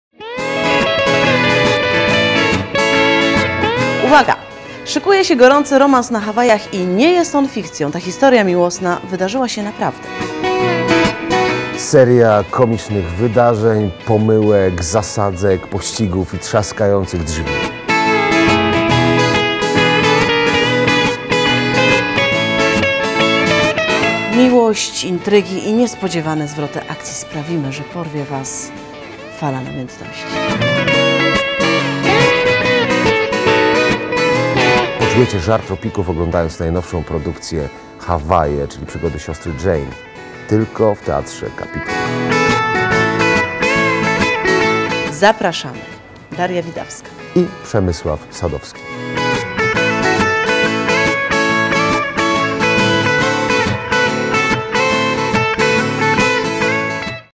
Hawaje, czyli przygody siostry Jane
"Hawaje, czyli przygody siostry Jane" to seria komicznych wydarzeń i zaskakujących sytuacji, pomyłek, trzaskających drzwi i historii z przeszłości tworzących nieprawdopodobny chaos. Dzięki głównej bohaterce uzbrojonej w dyktafon wszyscy wkoło zostają bohaterami gorącego romansu, który rozgrywa się w pięknej scenerii Hawajów.